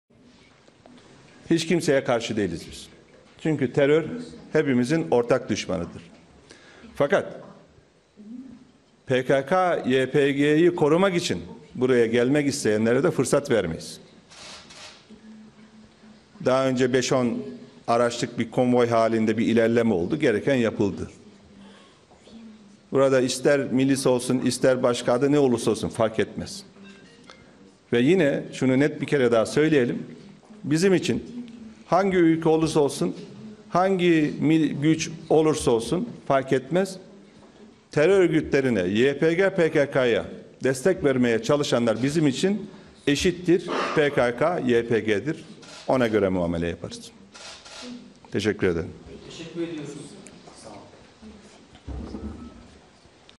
Çavuşoğlu'ndan Dikkat Çeken Açıklamalar
Çavuşoğlu: Terör örgütlerine destek verenlerin bizim için PKK/YPG'den hiçbir farkı yoktur!